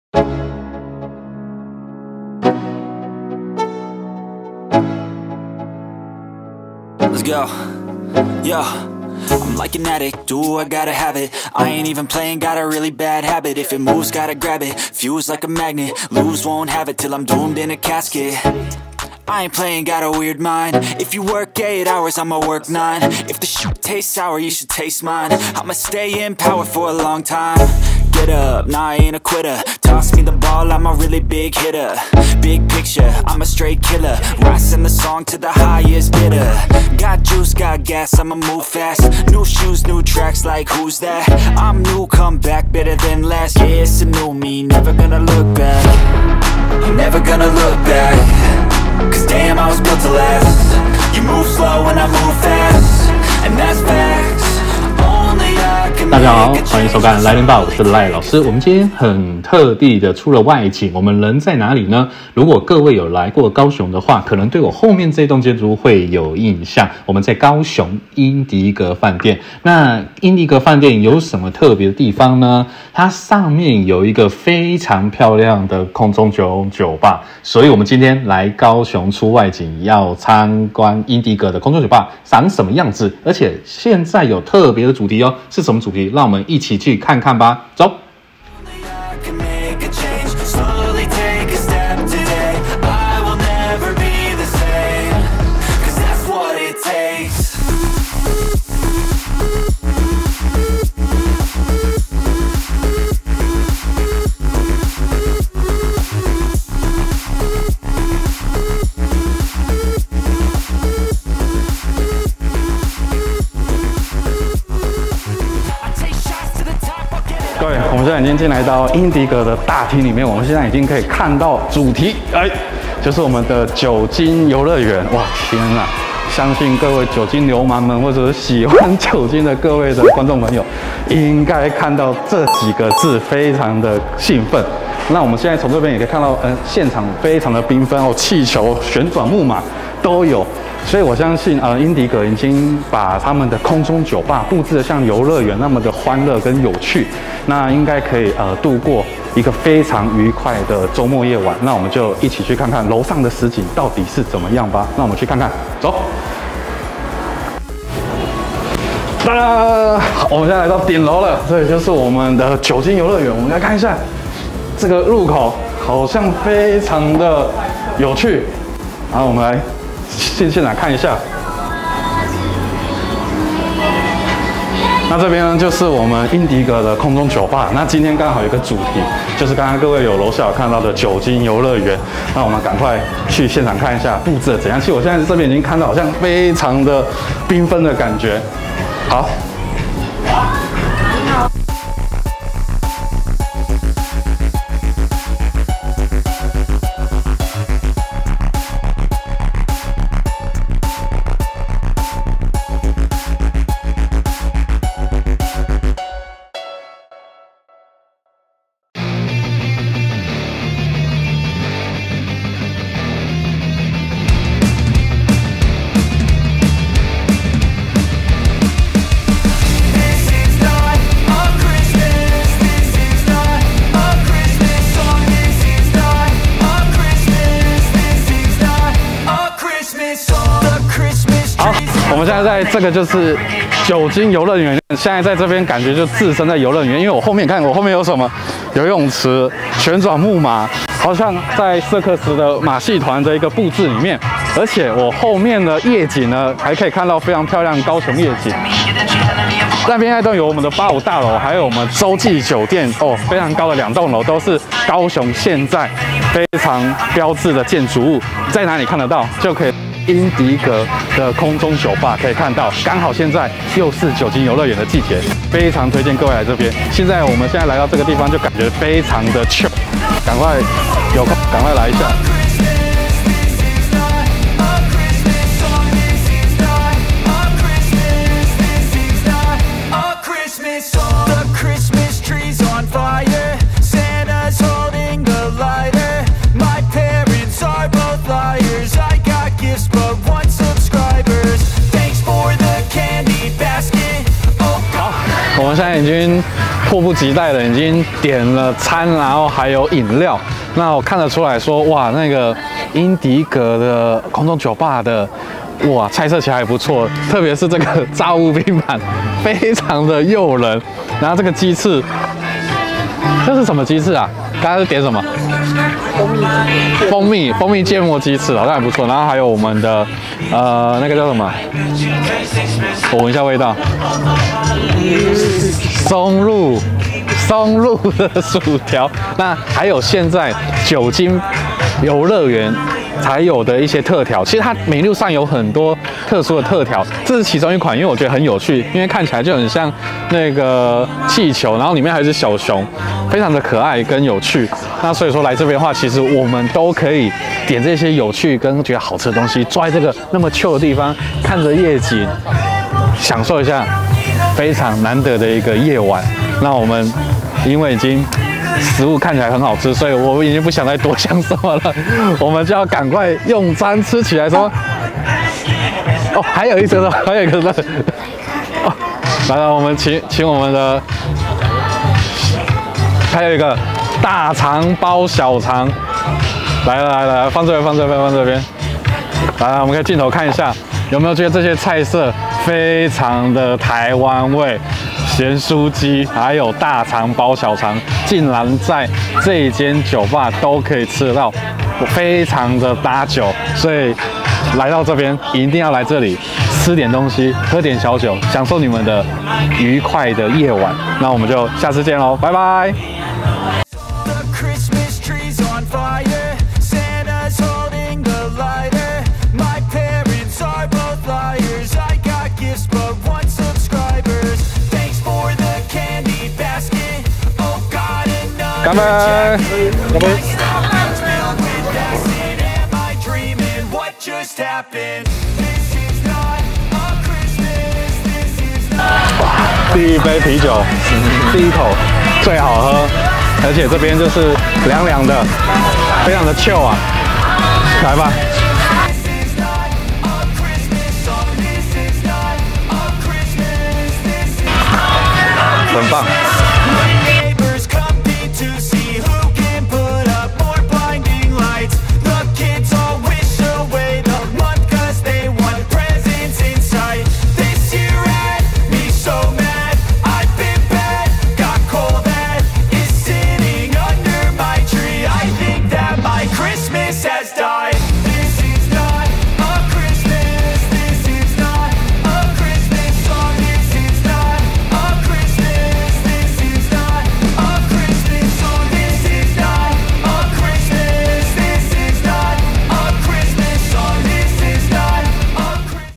酒精遊樂園 大人的樂園 高雄英迪格空中酒吧【LAILINBAR】-Cup38
這次我們來到高雄英迪格飯店的 空中酒吧-pier no.1
從3月開始一直到8/28 都有酒精遊樂園的主題！

空中酒吧佈置的非常童趣與繽紛，就真的像置身在遊樂園中。
加上活動的六款應景特調與餐點。怎麼能不來試一試呢！

而且到了夜晚，在英迪格酒吧 還可以看到美麗的高雄夜景哦！

非常推薦給各位觀眾 如果有去高雄旅遊～非常推薦來酒吧喝一杯吃點東西，享受一個美好的假期！！

此次拍攝特別感謝 英迪格飯店提供協助與幫忙。